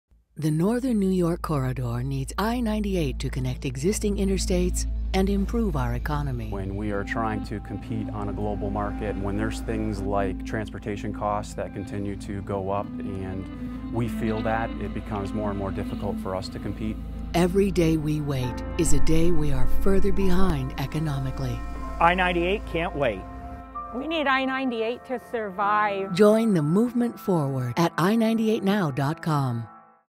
I-98 Now for Business
Interstate I-98 is needed, here's why...